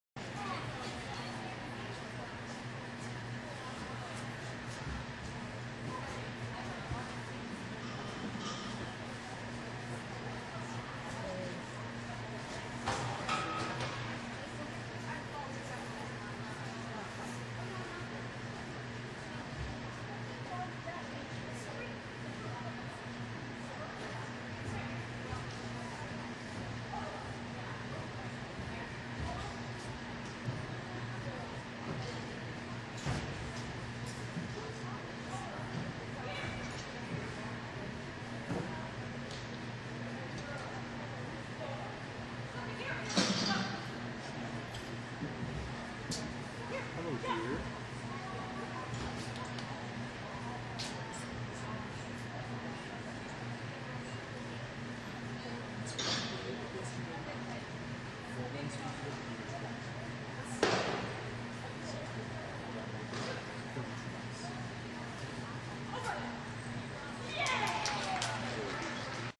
Sophie Bean Ralston's AKC Novice Standard run on Sunday January 10, 2010
Sophie's AKC Novice Standard run on Sunday January 10, 2010.  Great, near perfect run!